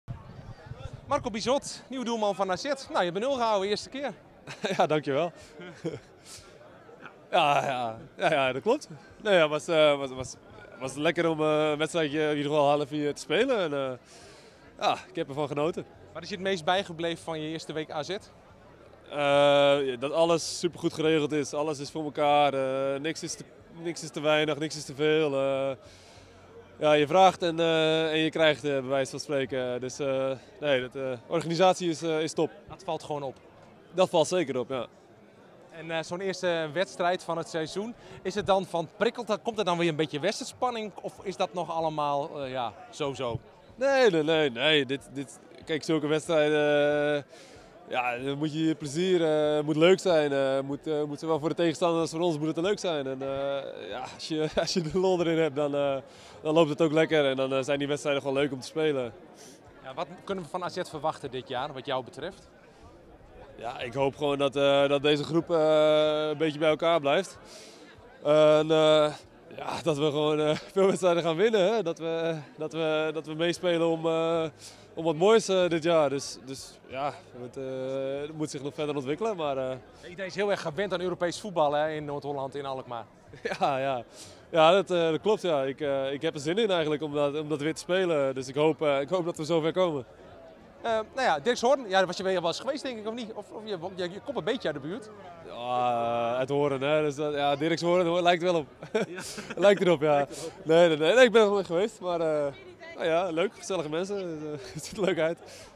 Reactie Bizot na Regioselectie - AZ
Debutant Marco Bizot verscheen na afloop van Regioselectie - AZ (1-7) voor de camera van AZ TV.